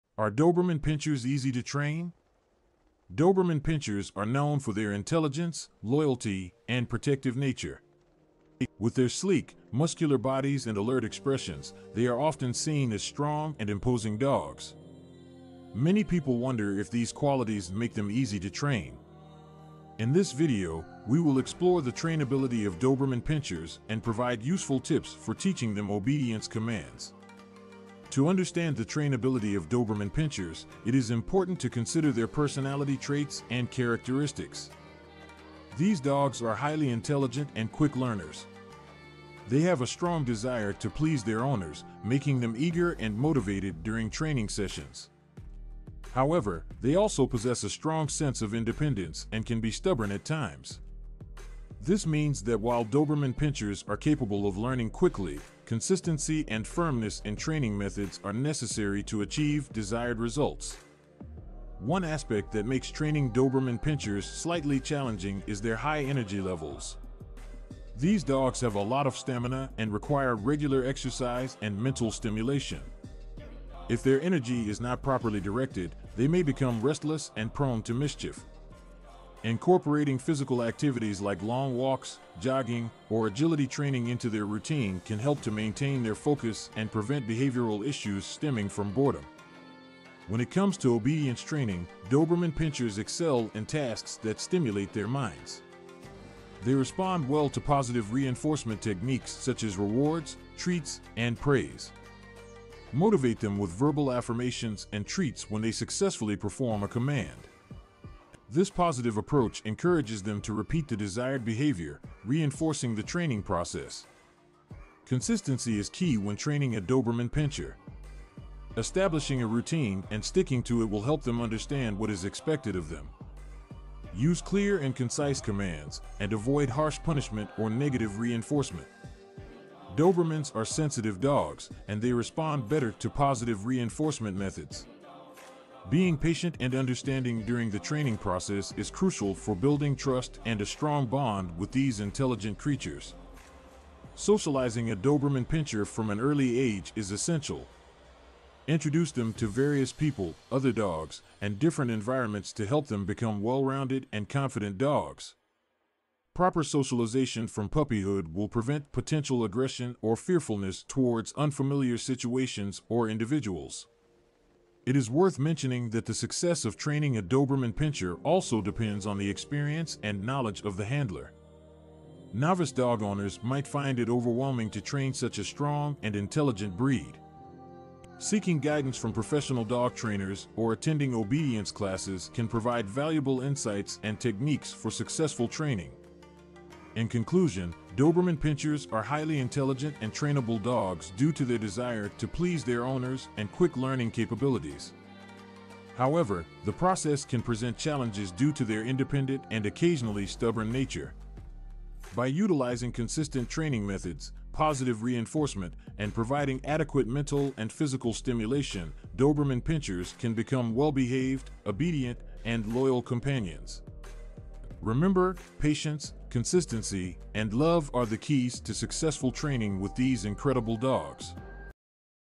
Train Your Doberman Pinscher: Tips for Obedience Training
Doberman Pinschers are an intelligent, loyal, and protective breed of dog. They can be easily trained, but require consistent reinforcement and positive reinforcement. Keywords: Doberman Pinschers, trainability, obedience commands, intelligent, loyal, protective, positive reinforcement.